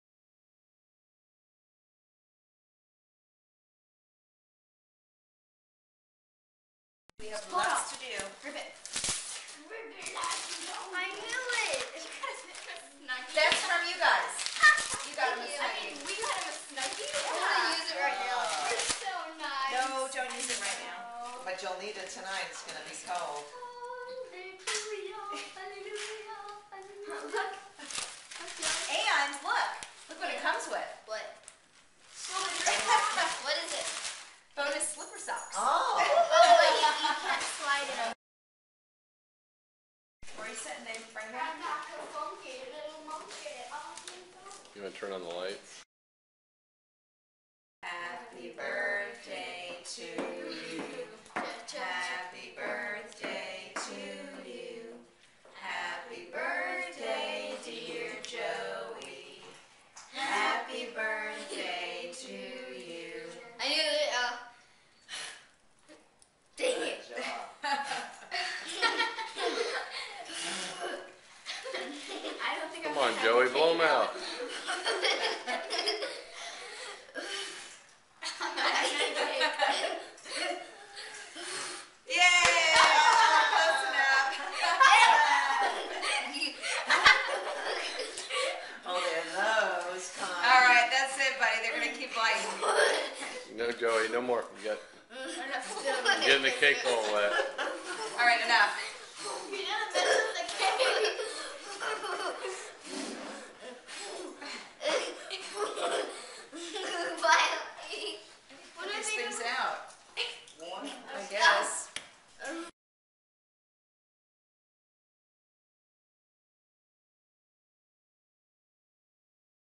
Joey Manausa 7 Years Old
Family celebration of Joey Manausa's 7th Birthday.